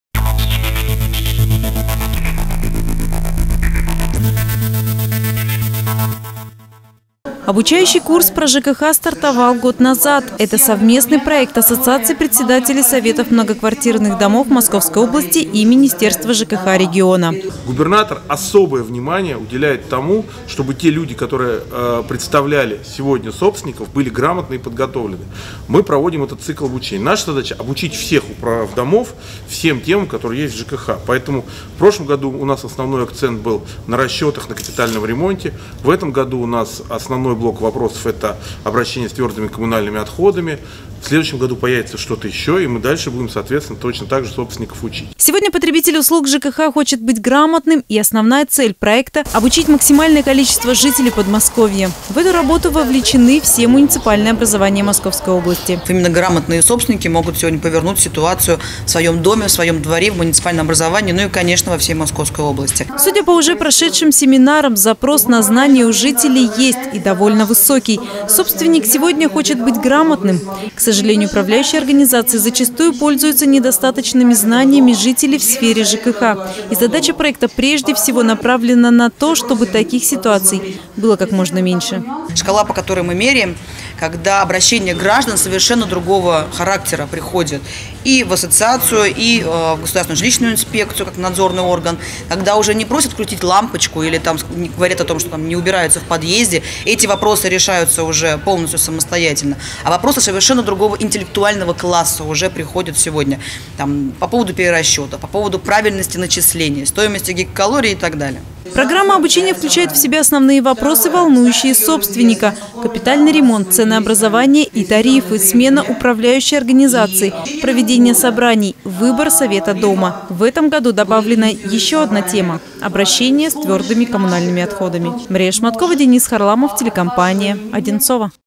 Проект «PROЖКХ» шагает по Подмосковью
В Одинцовском кампусе МГИМО прошел областной семинар. В числе спикеров – министр ЖКХ региона Евгений Хромушин. Слушатели – неравнодушные собственники многоквартирных домов.